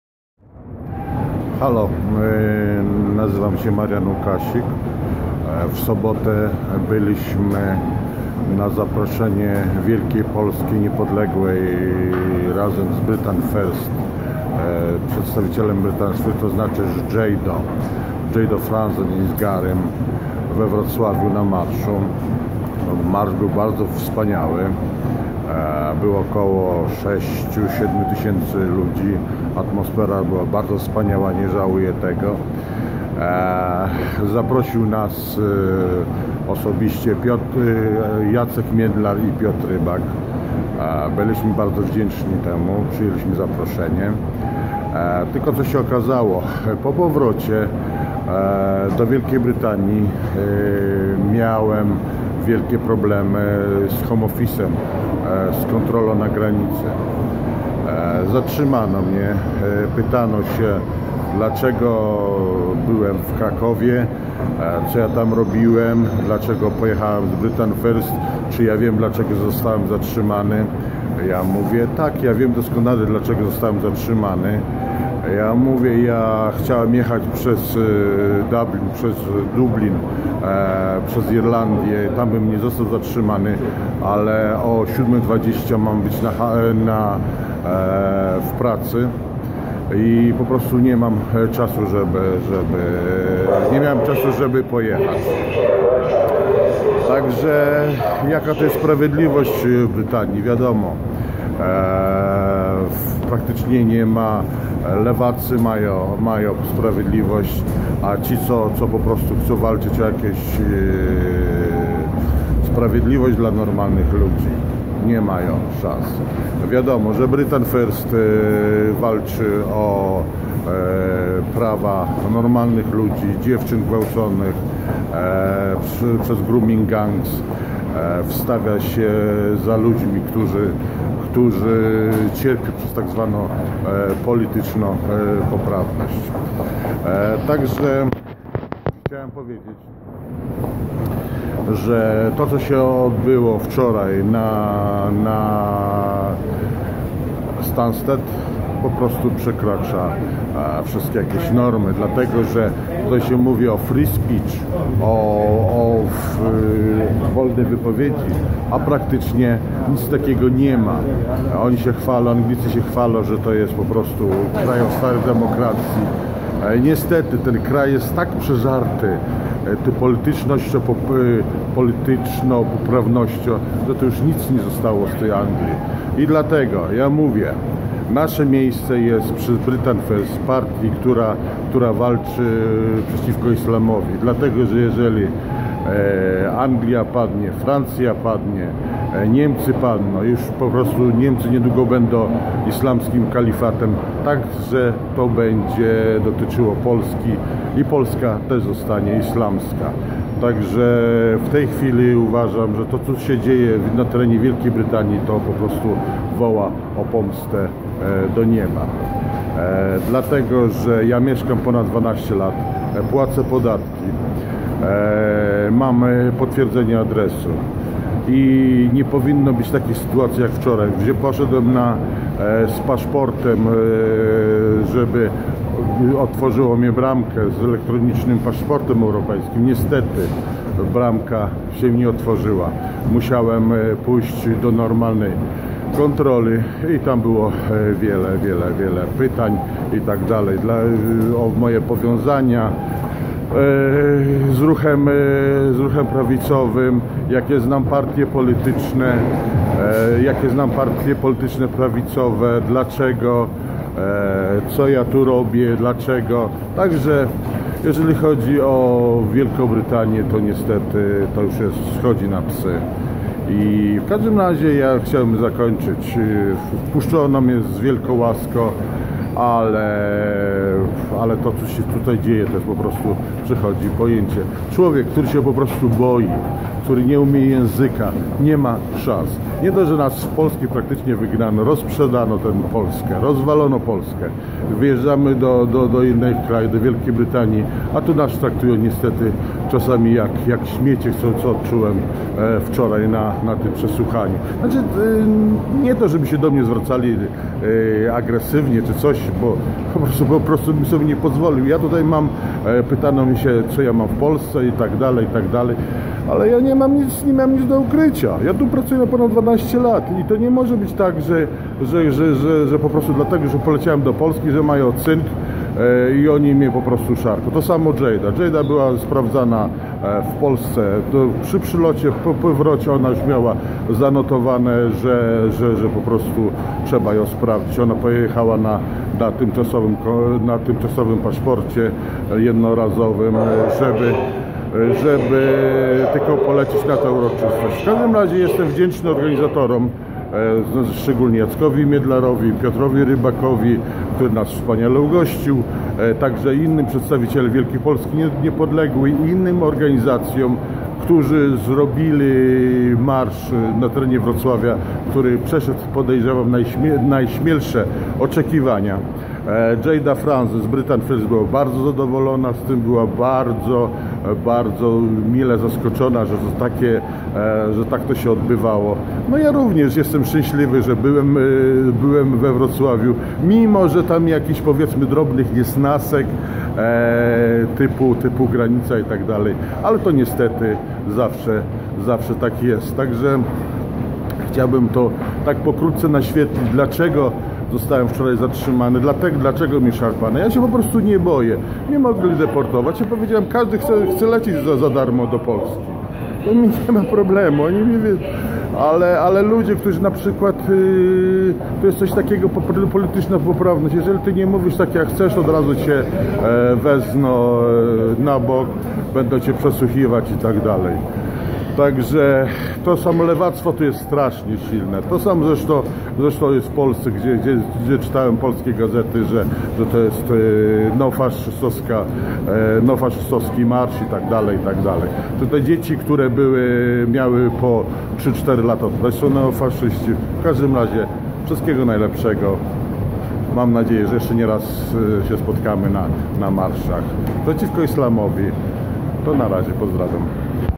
Dlaczego Home Office nie chcial mnie wpuscic do UK?
Deportacja z UK za udzial w Marszu Niepodleglosci?
Nazywam sie Marian Lukasik-na zaproszenie Jacka Miedlara i Piotra Rybaka udalismy sie do Wroclawia na 99 rocznice Swieta  Niepodleglosci.

Why British Border Control wanted stopped me?

Organizatorzy  przyjeli  nas wspaniale wszystko mielismy zaplacone-Jayda byla gosciem honorowym Marszu.
Marsz zaczol sie 11 listopada 2017 zprzed  Dworca Kolejowego we Wroclawiu Glownym az do Rynku na Starym Miescie.
Podczas calej imprezy nastroj byl wspanialy pomimo padajacego deszczu ale nikt sie nie poddawal I maszerowal wszyscy dalej dzielnie.
Zgrzyt nastapil wtedy kiedy lewacy probowali zaklocic marsz, probujac  wszczac burdy ale to im udalo sie w malym stopniu-demonstracje I antife przegradzali policjanci w pelnym rynsztoku bojowym.
Marsz zgromadzil okolo 5 do 6 tysiecy ludzi-jak prasa proniemicka probowala przedstawic napewno nie byla to jak napisal Onet.pl-neonazistowska legalna demonstracja.
Kto byl faszysta?- chyba temlode kobiety I dzieci w wozkach
Na Rynku z samochodu  przemawiali miedzy innymi Jacek Miedlar,Piotr Rybak I Jayda Fransen z tlumaczeniem na polski  przez Jacka Miedlara.
Bylem  bardzo zbudowany postawa uczestnikow marszu-wiekszosc stanowili kobiety I dzieci.Nie zabraklo takze ludzi w starszym ktorym niejednokrotnie trzeba bylo pomagac w marszu.
Zgrzyt nastapil na lotnisku we Wroclawiu gdzie Jayda byla doprowadzona na kontrole osobista z powodu wpisu jaki zostal wyslany z Wielkiej Brytanii do wszystkich lotnisk europejskich a chodzilo o to ze jest bojowniczka walki z Islamem a to bardzo nie podoba sie wladzom brytyjskim.
Lot do podlondynskiego Stansted  odbyl sie bez problemow.
Horror dla mnie zaczal sie na Stansted dlatego,ze bramka  nie otworzyla sie –mialem  nadzieje ze sie otworzy.
Pracownica lotniska polecila mi udac sie do funkcionariusza  Boder Control niestety nie zostalem przepuszczony-w eskorcie straznika granicznego udalem sie do odgrodzonego pomieszczenia gdzie mialem oczekiwac na decyzje.
Zaczelo sie maglowanie poproszono mnie o karte wstepu na poklad oraz zaczelo sie zadawanie pytan.
Pierwszym pytaniem dlaczego bylismy  we Wroclawiu na Marszu Niepodlegosci –dlaczego podrozowalem w towarzystwie Jaydy Fransen z Britain First.
Wszystko wiedzieli na temat  Jacka  Miedlara I Piotra Rybaka
Funkcjonariusze wiedzieli wszystko dlatego tez mowilem prawde.Mysle,ze byly podsluchiwane rozmowy I wiedzieli  wszystkie szczegoly.
Funkcjonarusz  spytal  sie mnie –czy  bede mial gdzie mieszkac gdy bede deportowany do Polski-pomyslalem,ze deport mam w kieszeni.
Powiedzialem im,ze podam ich do sadu jak mnie deportuja!!!
Kontroweryjny  wywiad  na temat  Angeli Merkel  od 15.43 minuty do 16.40
Dzisiaj dowiedzialem sie o aresztowaniu Piotra Rybaka na miesiac I 20 dni-straszne to lewactwo ma takie silne uklady I pieniadze
Pozdrawiam
Marian Lukasik